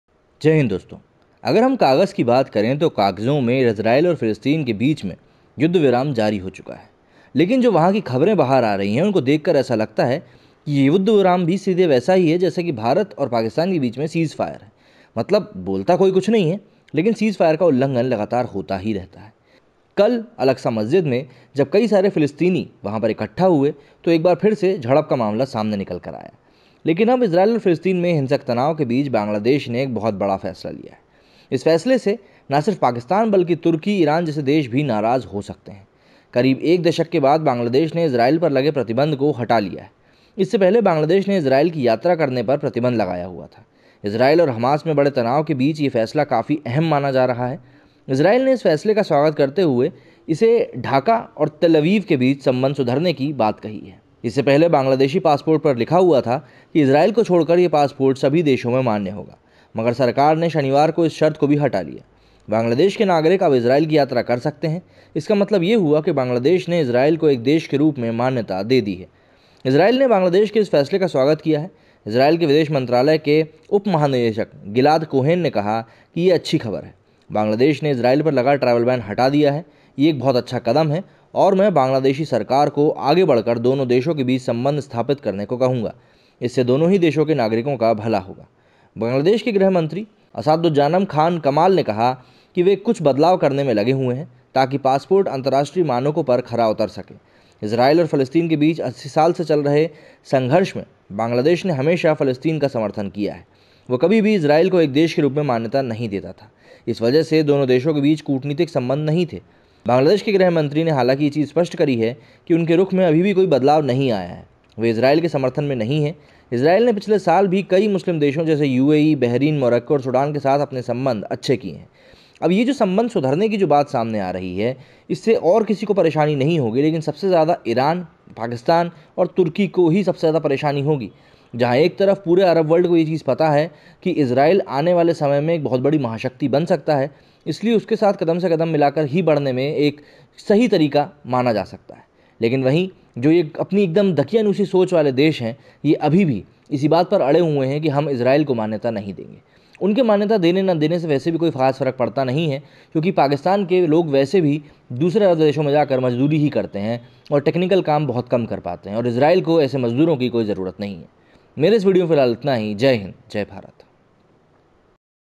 बांग्लादेश के इस काम से पाकिस्तान को लगी मिर्ची Israel के लोगो ने कहा शुक्रिया। Naman Sharma
Stay Blessed